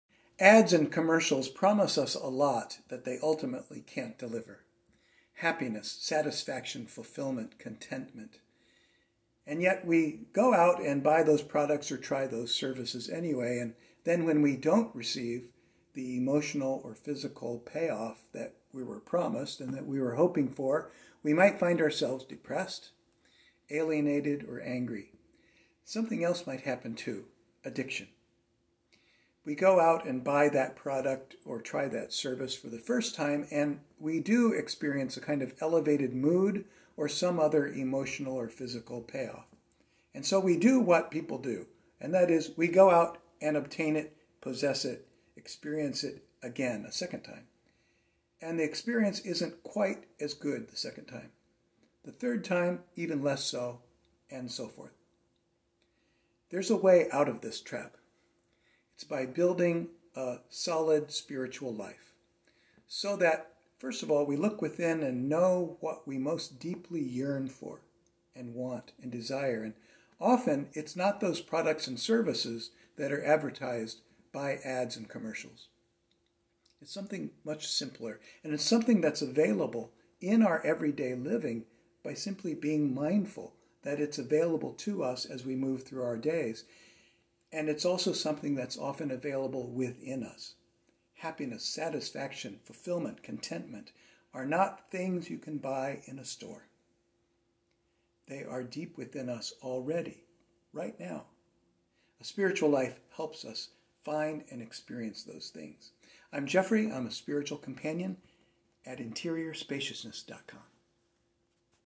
Addiction, too
Chasing the false promises of advertisements and commercials contributes to depression and addiction.